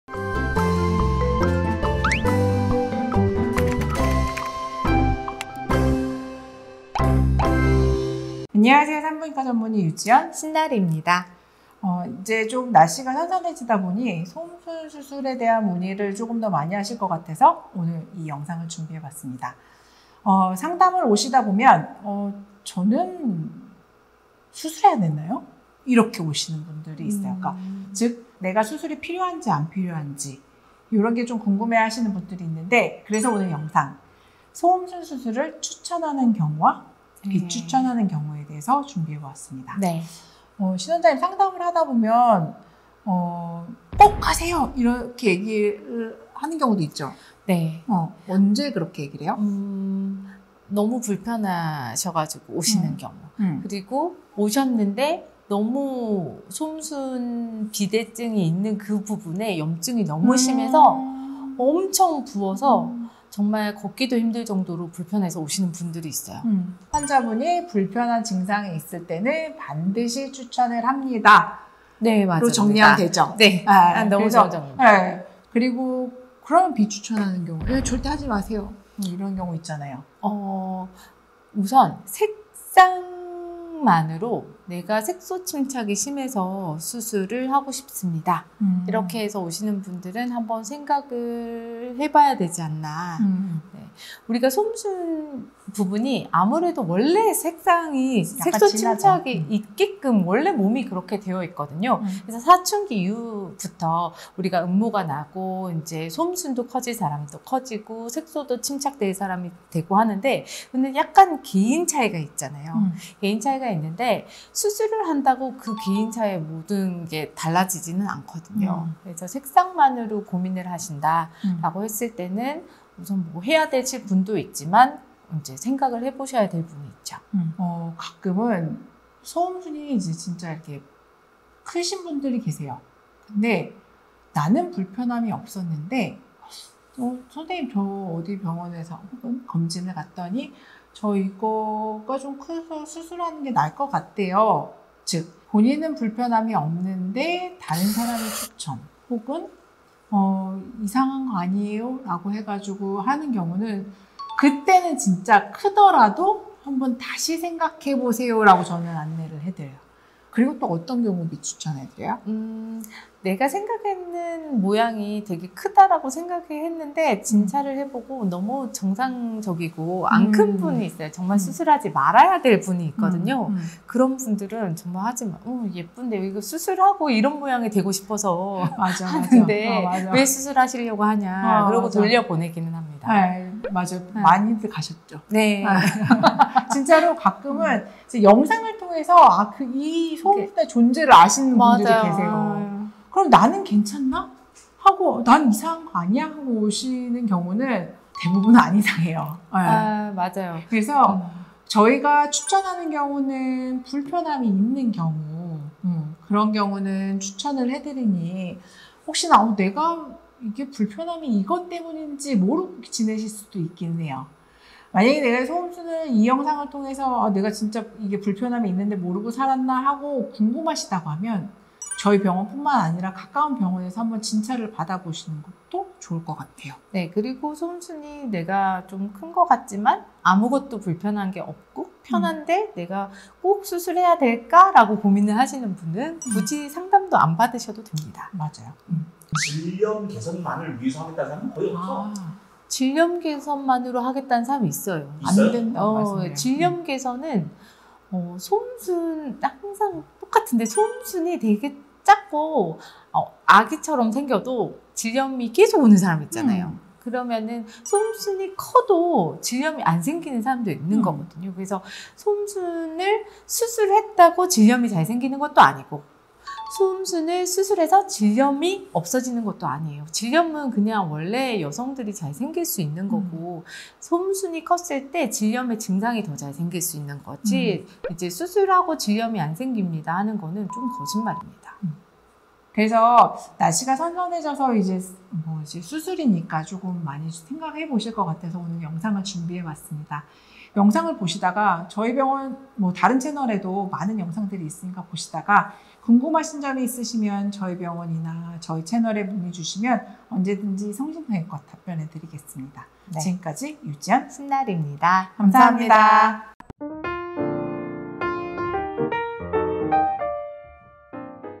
추천 vs 비추천 (ft.소음수술을 고민하고 계신분들 필수영상)
소음순 수술 해야될까? 안 해도 될까 ? 를 고민하시는 분들에게
소음순 수술 많이하는 더끌림산부인과 대표원장 유지연 신나리 원장님이 알려주시는
소음순 수술
[추천 케이스 VS 비추천 케이스]
소음순 수술을 망설이시거나 계획중이신 분들 꼭 시청해 주세요~~~

여러분의 여성건강은 물론이고 여성의 아름다움까지 책임지고자 최선을 다하고 있는
더끌림산부인과 입니다.